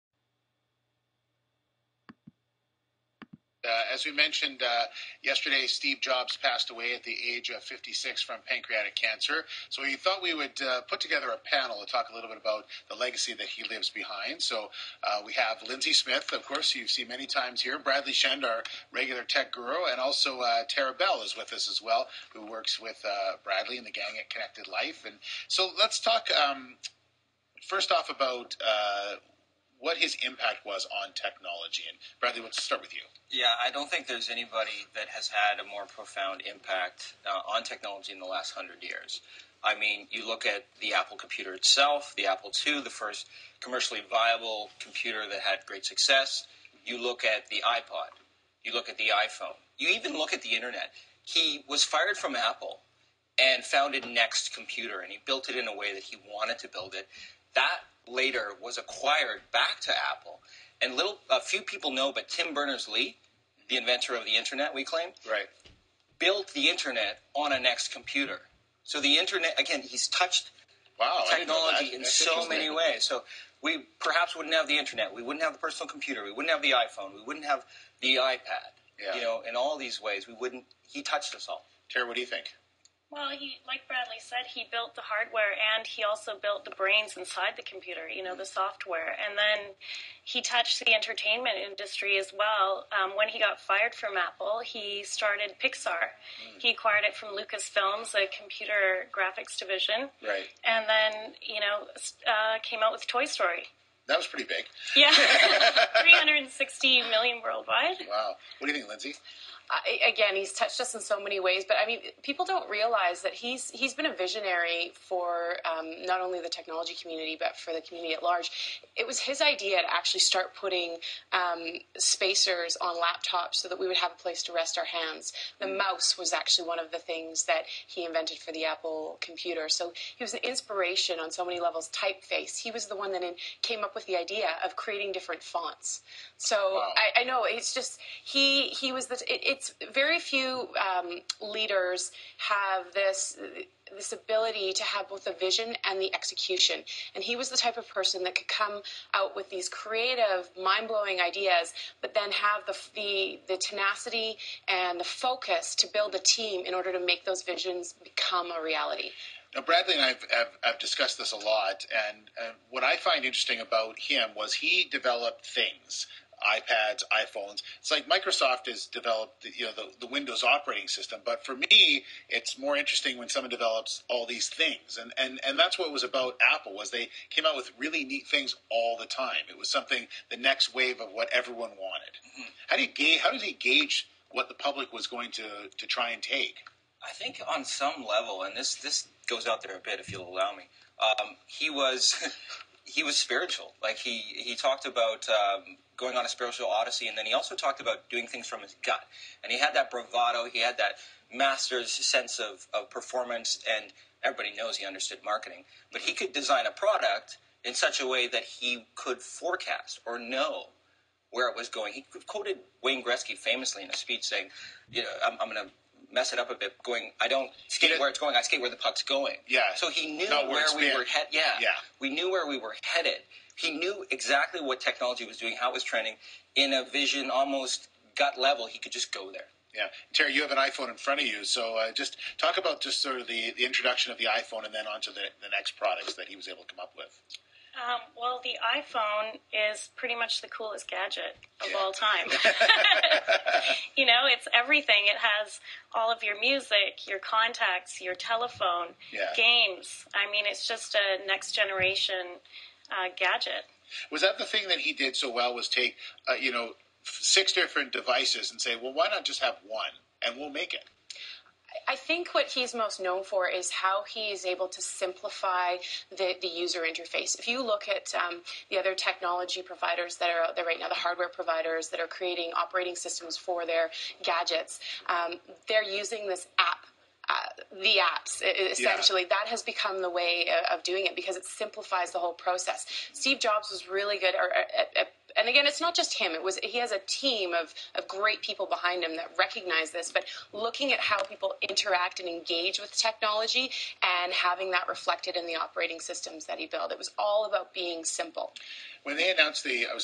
Remembering Steve Jobs, Founder of Apple - Part 1
Global BC TV Morning News segment with Steve Darling. Talking with Lindsay Smith (TechLinz), Bradley Shende, and Tara Bell about the legacy of Steve Jobs.